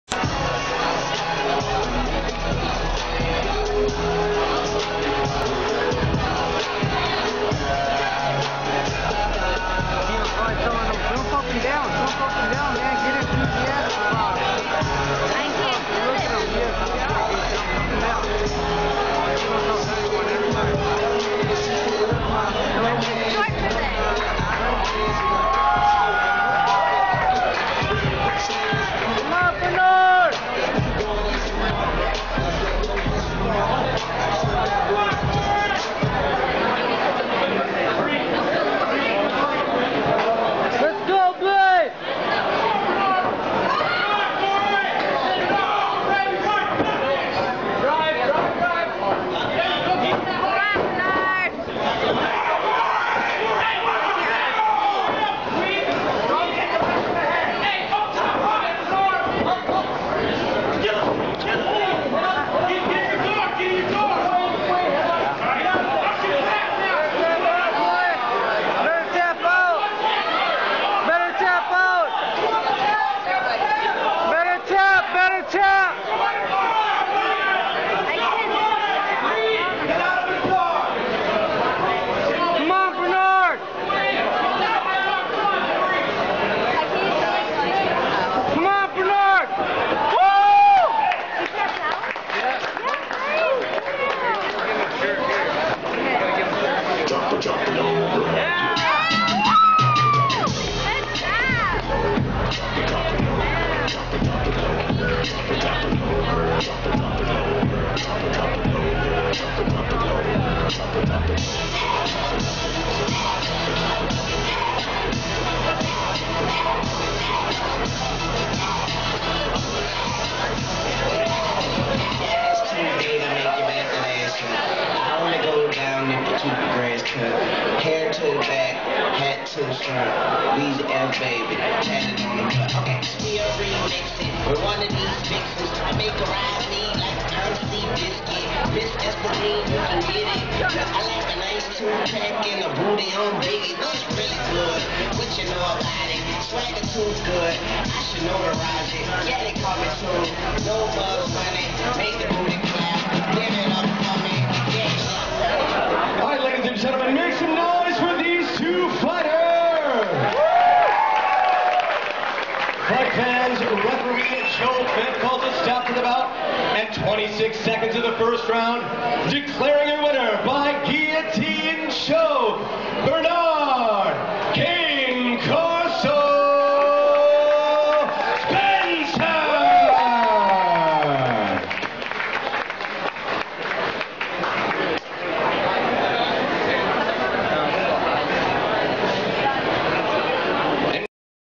GUARD DOG MMA "Cane Corso"
this was his first fight in Minnesota GREAT JOB BERNARD we love you man